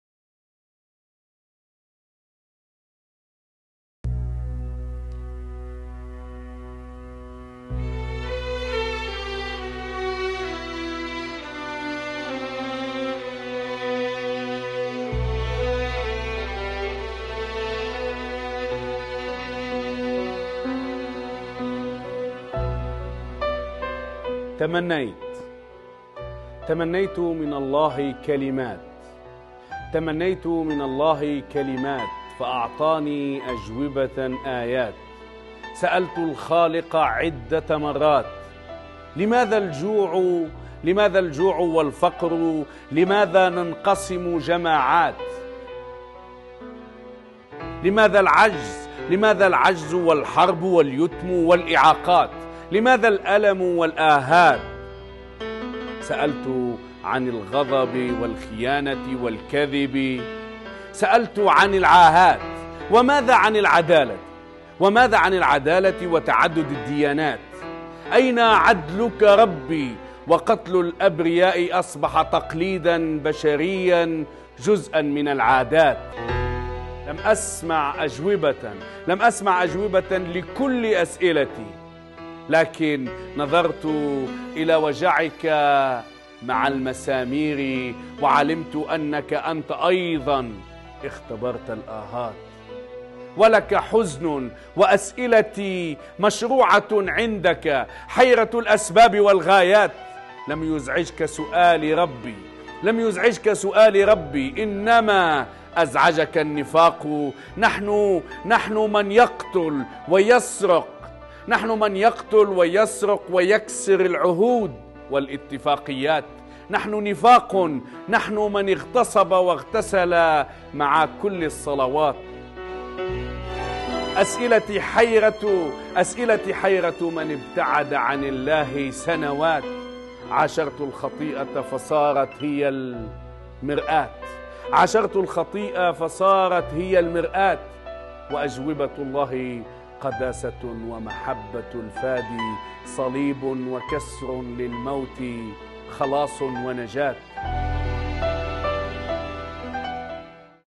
Imed's Poetry
Christian poetry written by the presenter of our live program called "Forbidden" - Imed Dabbour. Imed is Tunisian and the accent of his program is on the current affairs in North Africa and everything concerning the Christians living in the region.